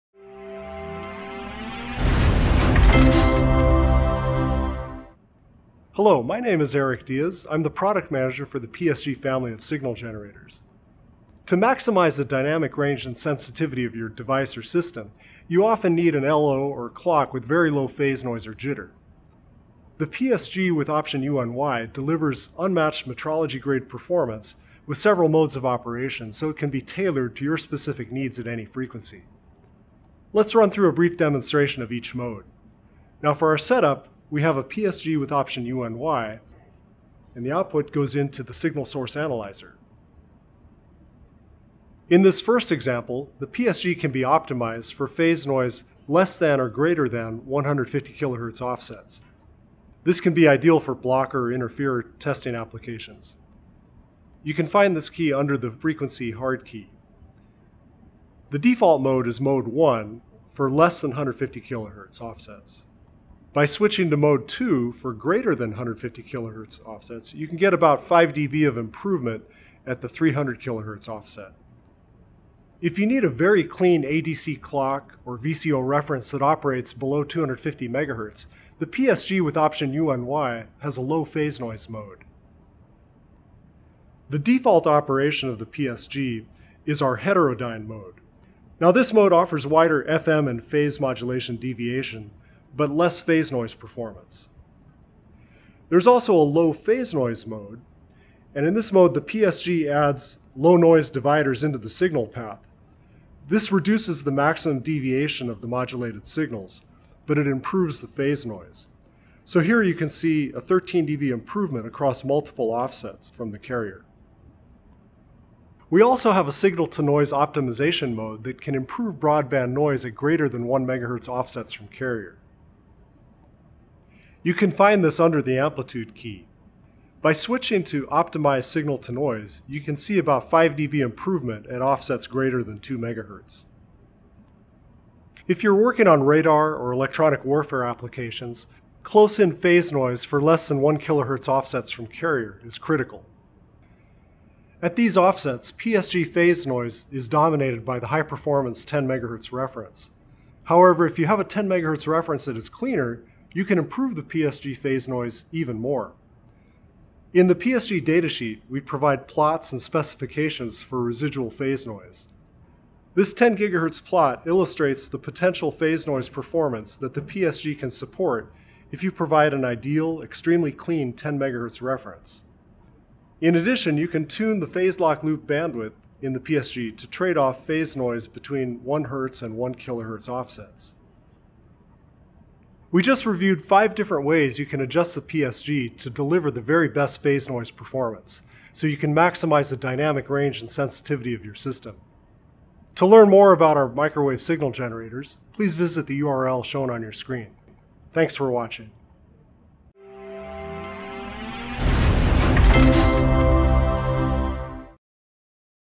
Achieve Even Lower Phase Noise | PSG Signal Generators | Keysight Technologies
To maximize the dynamic range and sensitivity of your system, you need an LO or clock with very low phase noise as well as the flexibility to tune it to different frequencies. Keysight PSG signal generators with Option UNY deliver unmatched metrology-grade phase noise performance with several modes of operation that can be tailored to your specific needs at any frequency. Discover five modes that improve the already low phase noise of the PSG.